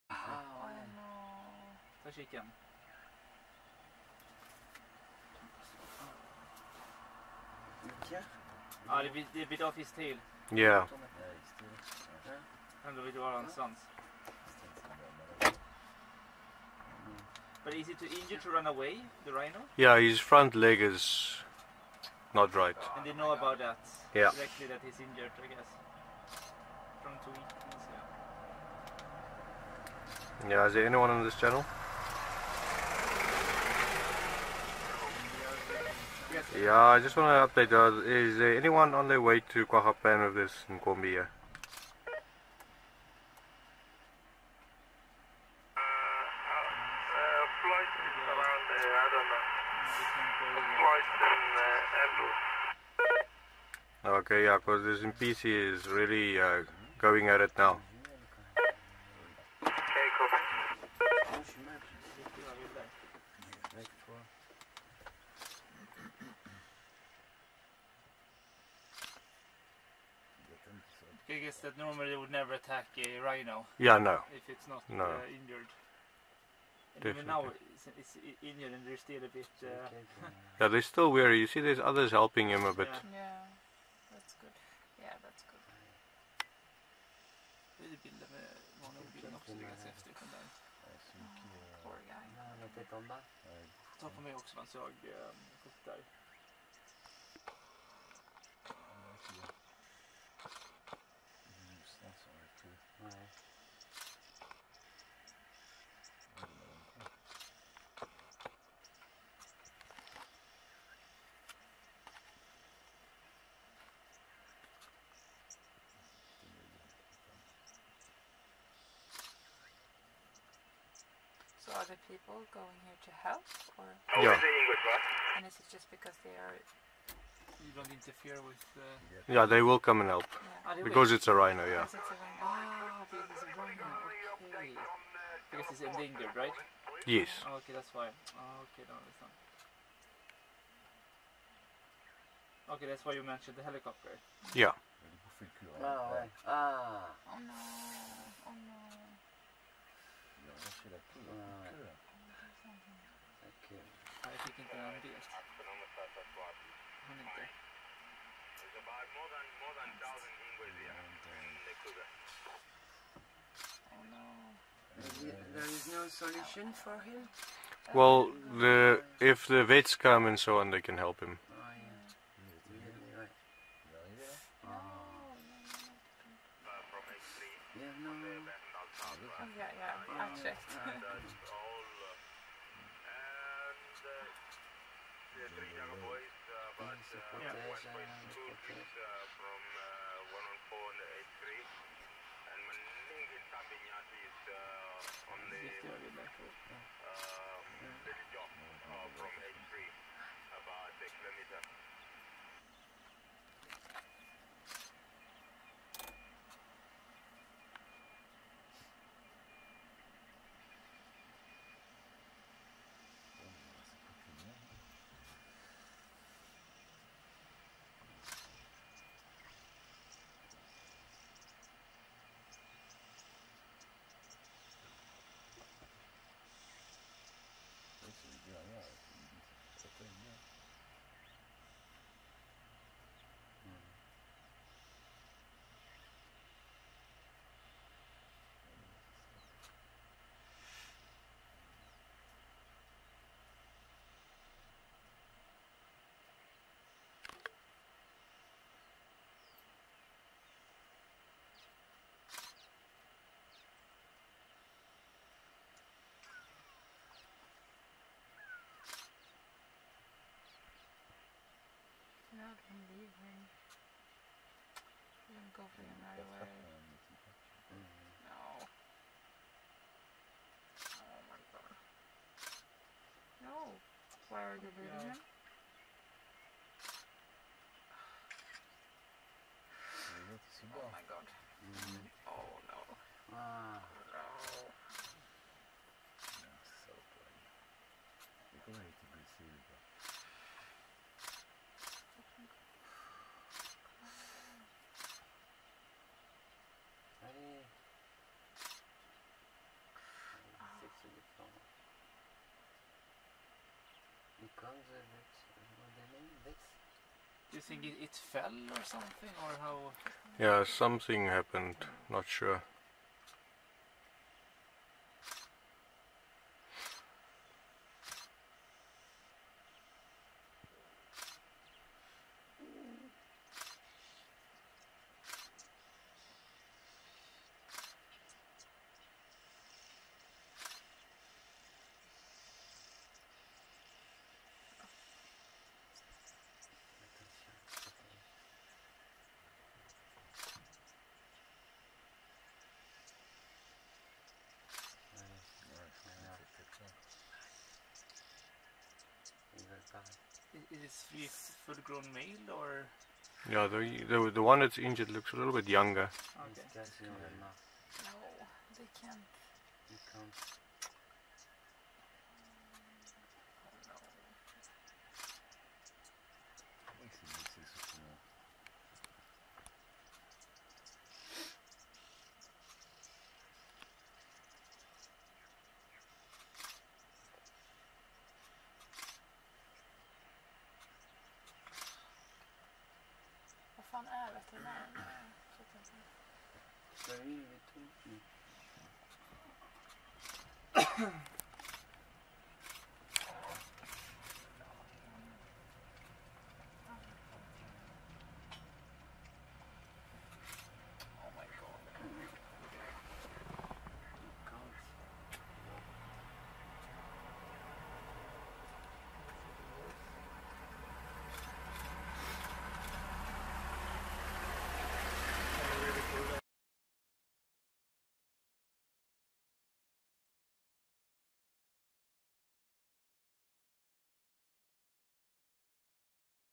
Hyenas Vs Rhinos At Kruger National Park || WooGlobe
In this Hyenas Vs Rhinos video, there were these 3 rhinos busy drinking water when the hyenas came into the area, we noticed that the one rhino had a broken leg and couldn't walk properly. And so the hyenas also noticed it. The started biting the rhino on the back where the tail is, and in that tore of the tail. Hyenas kept biting and the rhinos kept defending him. They later ran down towards the river bed where we lost sight. News came back that the rhino have died of his injuries later that day.

Copyright - WooGlobe.